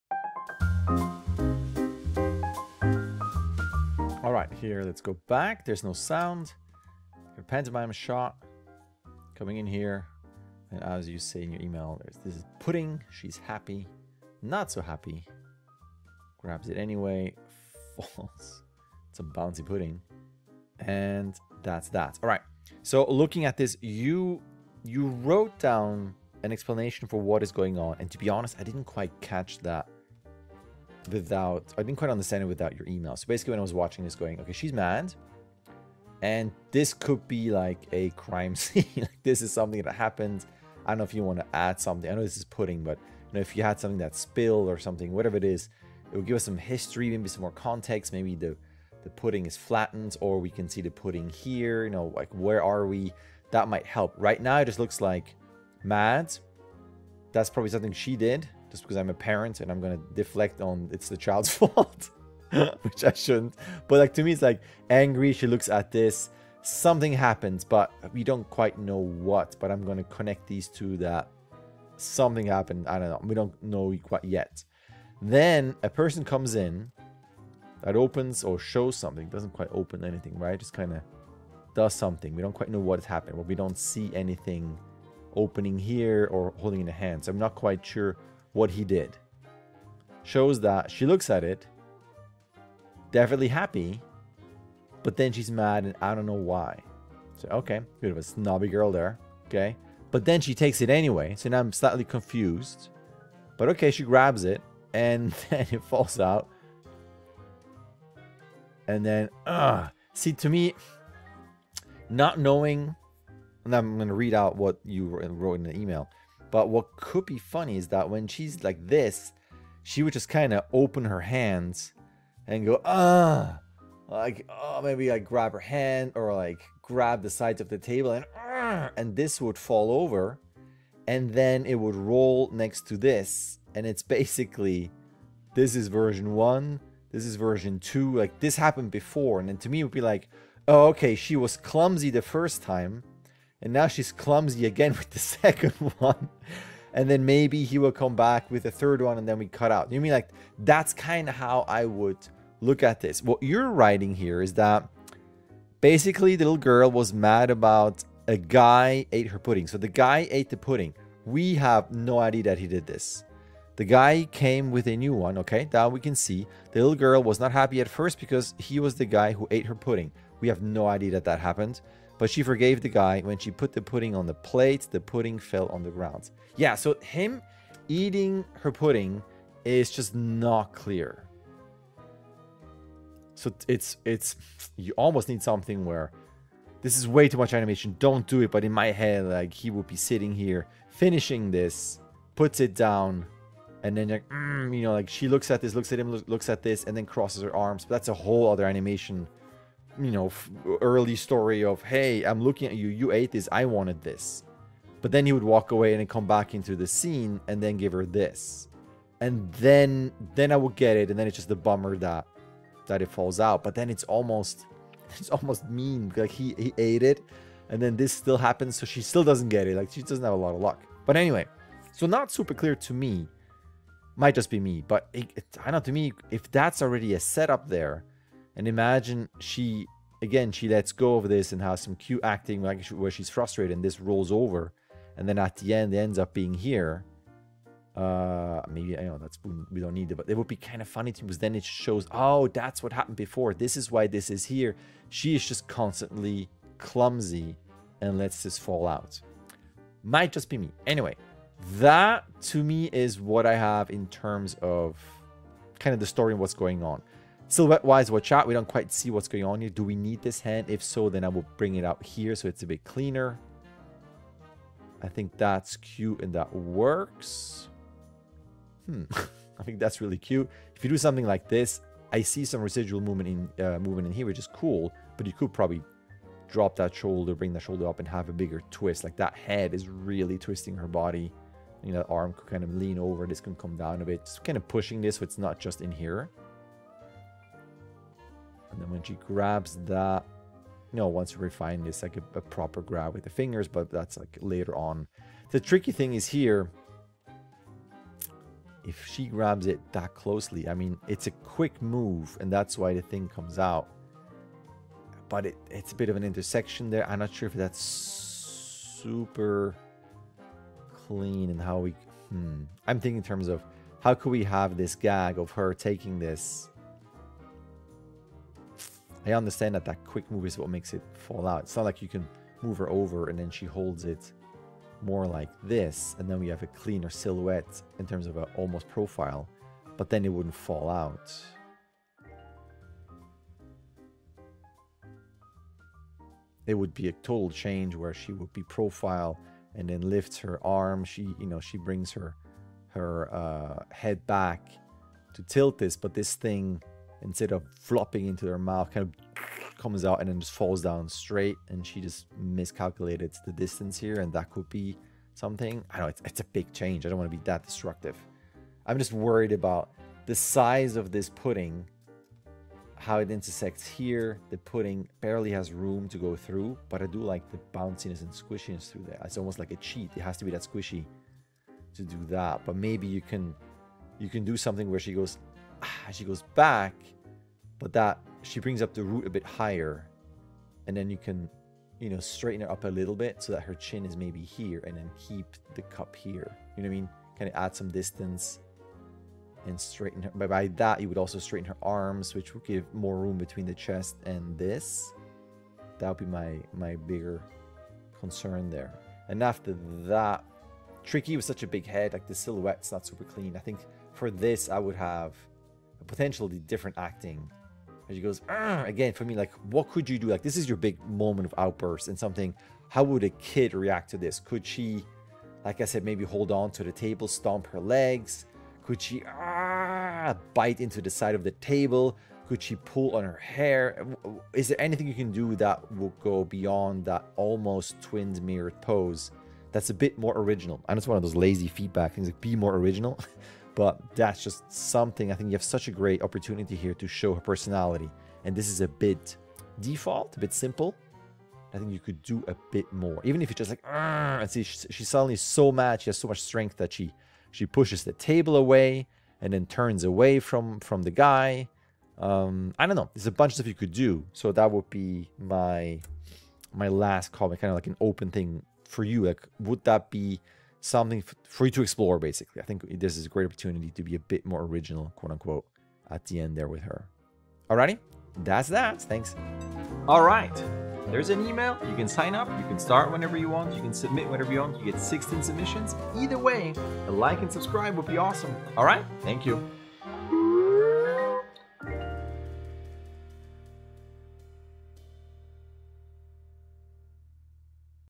Animation Workshop Feedback - Anonymous 1a (2023)
You're watching a feedback session for the Spungella Animation Workshop. Topics that are covered: lipsync, facial animation, body mechanics, acting, maya animation.

✔️ Animation Director, Lead, Animator || Blizzard Cinematics, Paramount/Nickelodeon, Warner Brothers Animation and Industrial Light & Magic (19 years)